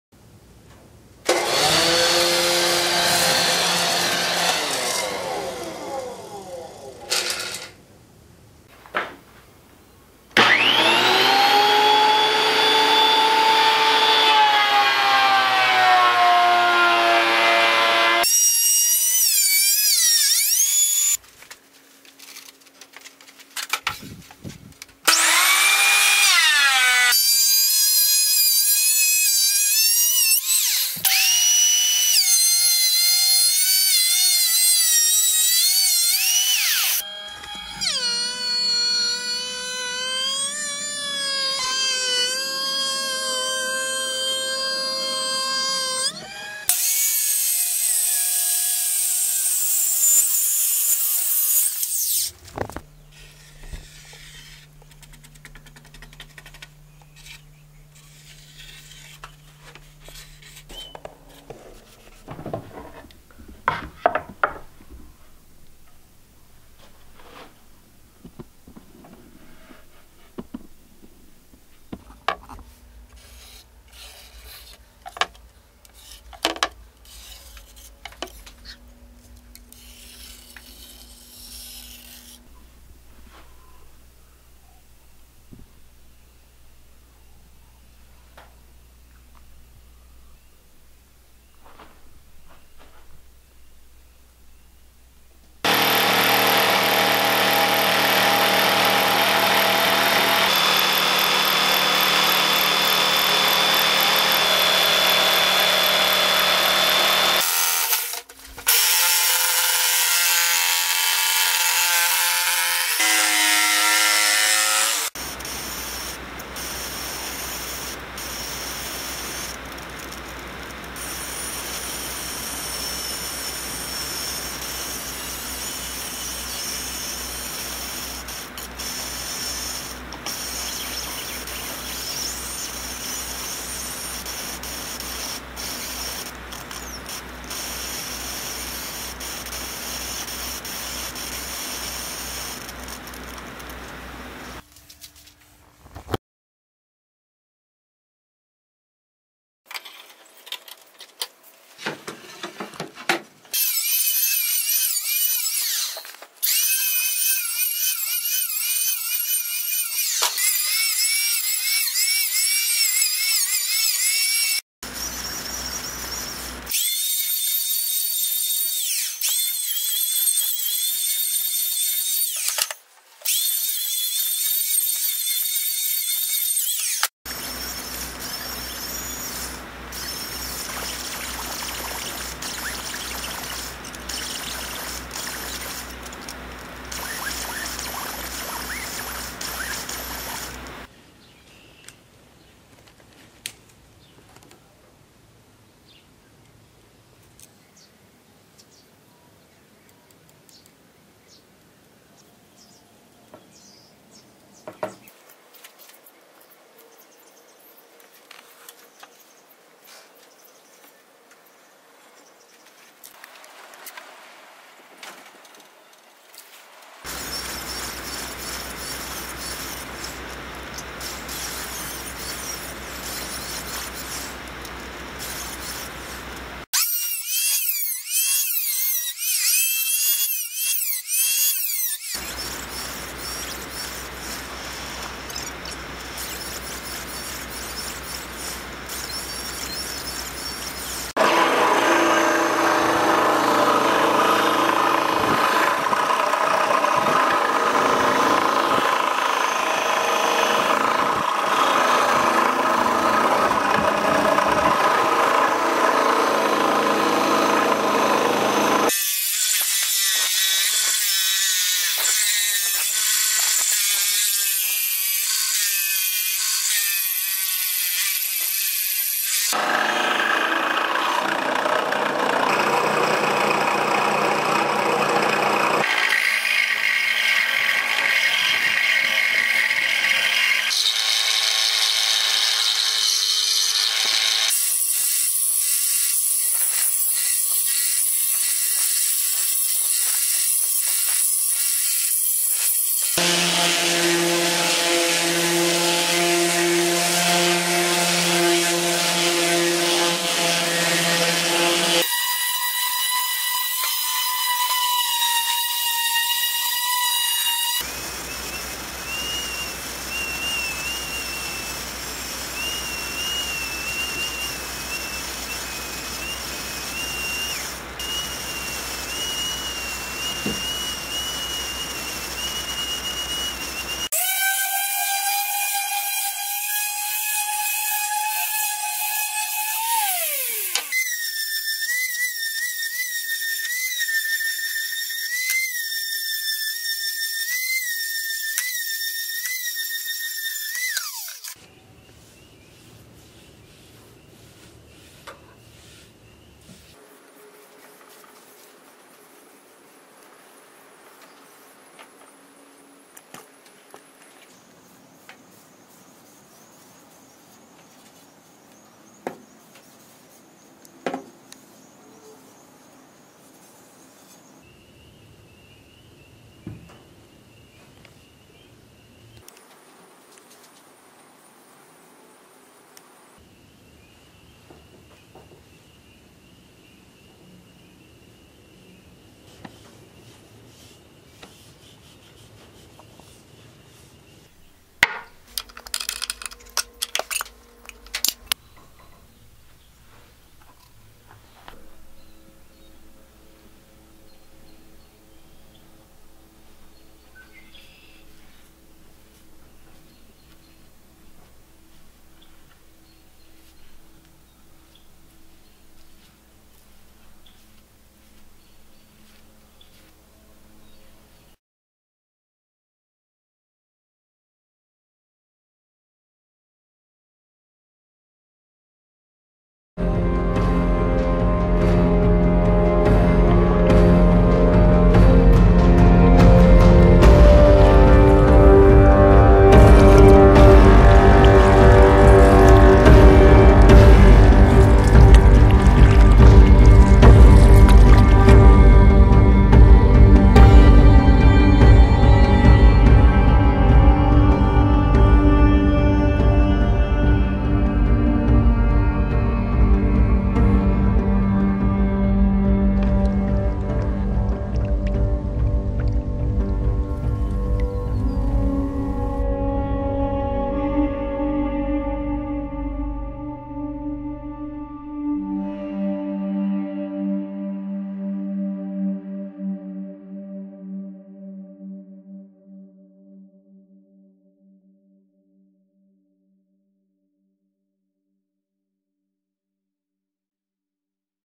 One-Piece Canoe Paddle
Made seven paddles from sassafras boards.